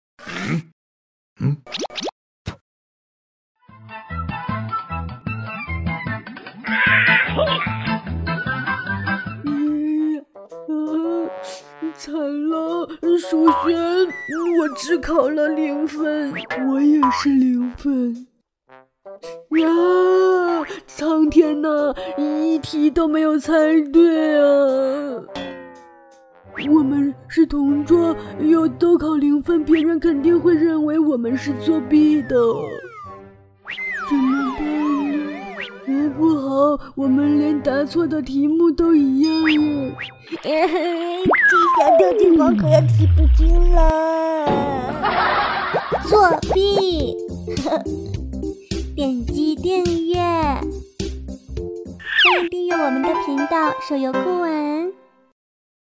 【愤怒的小鸟同人动画】作弊
我们的可爱猪朋友们，有烦恼了，他们并没有在考试中作弊，但是担心大家会怀疑他们，这是为什么呢？